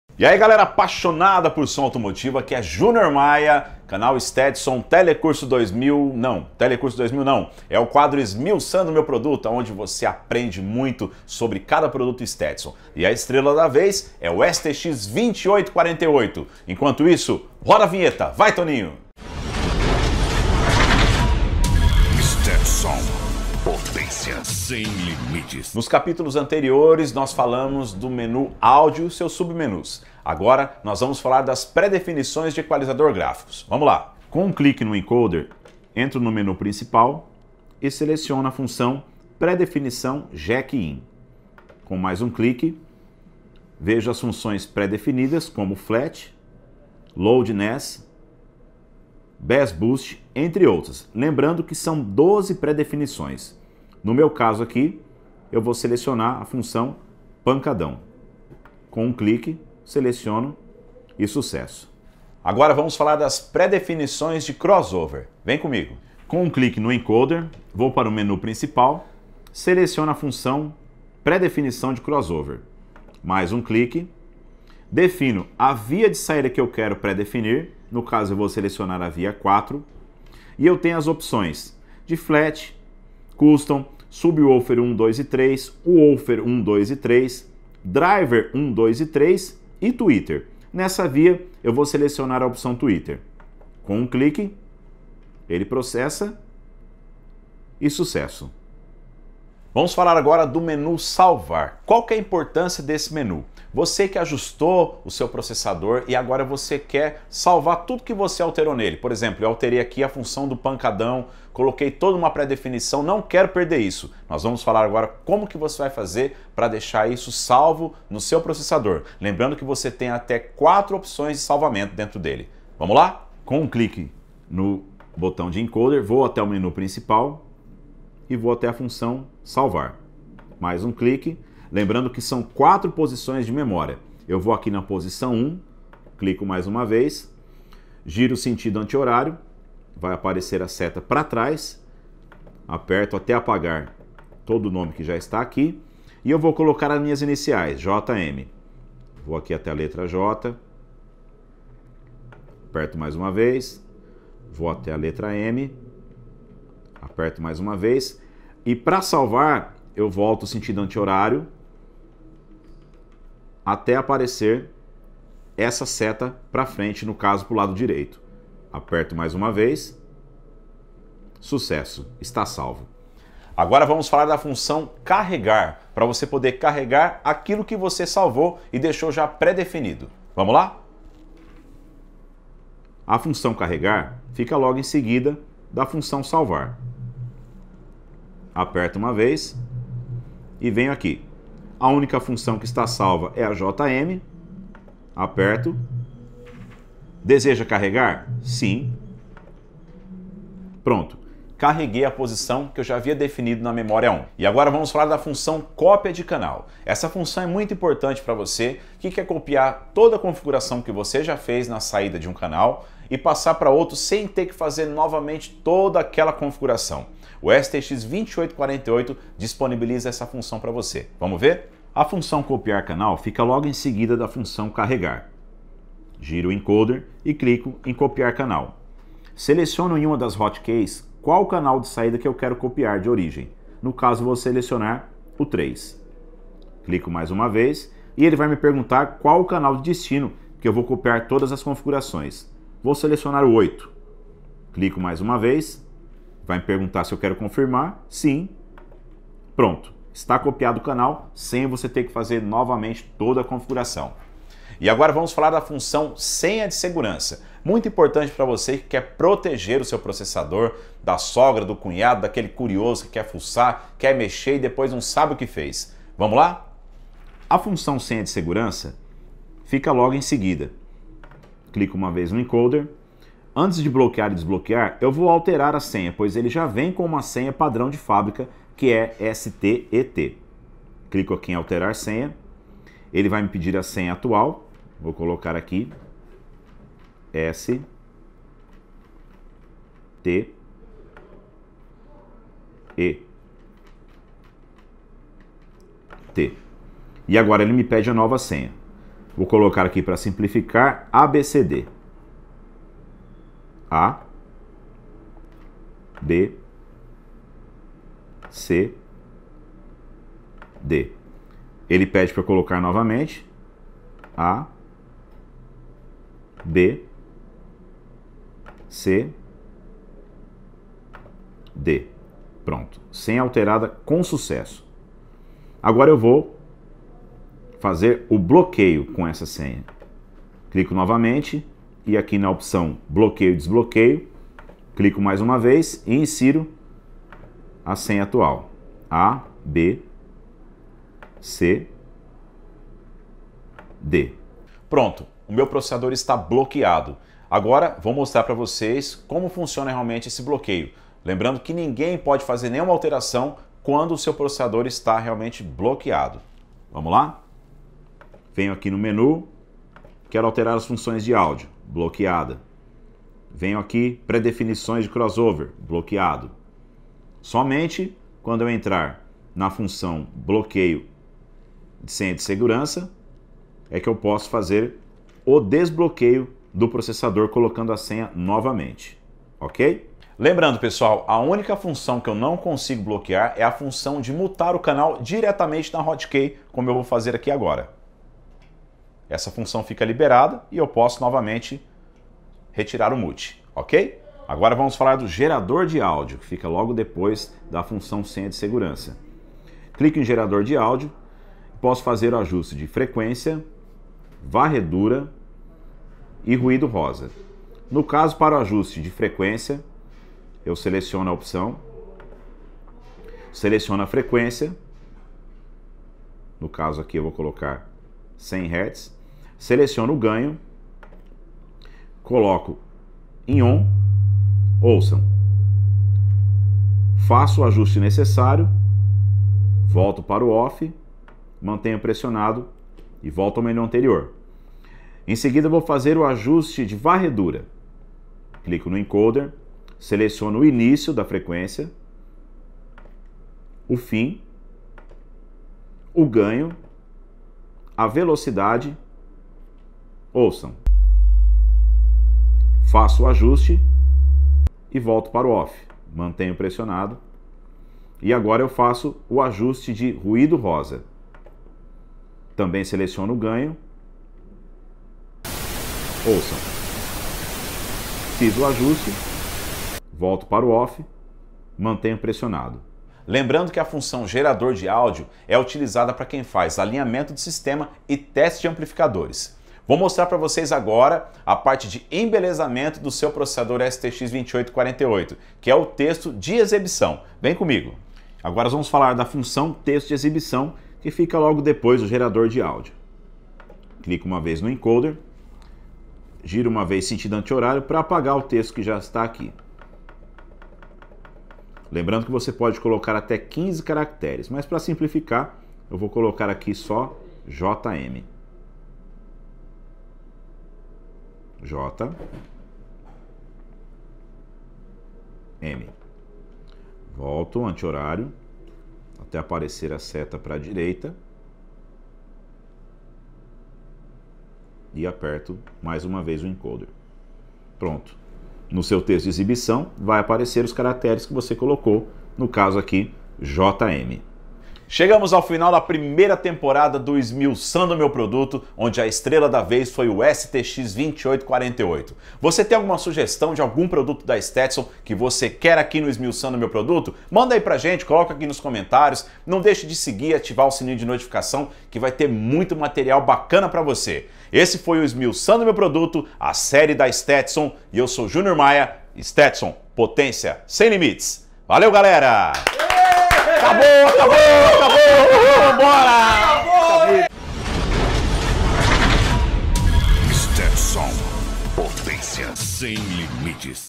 DIGITAL AUDIO PROCESSOR - STEP BY STEP (ep01) - CAR AUDIO DSP Crossover
Learn, step by step, how to configure the DSP Digitnal Signal Processors - STETSOM